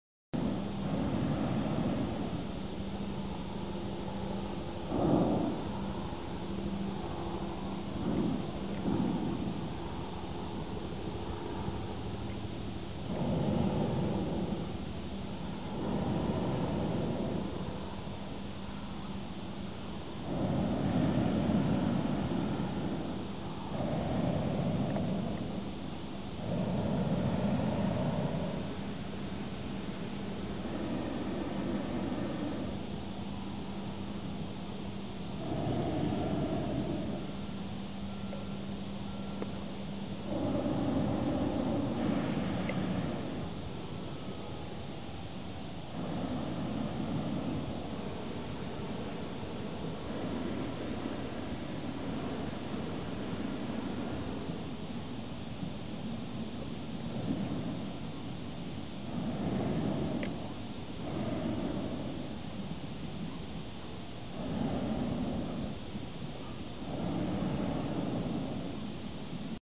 Balloons over the Rio Grande Bosque NM
Balloons over the Rio Grande New Mexico. Albuquerque International Balloon fiesta.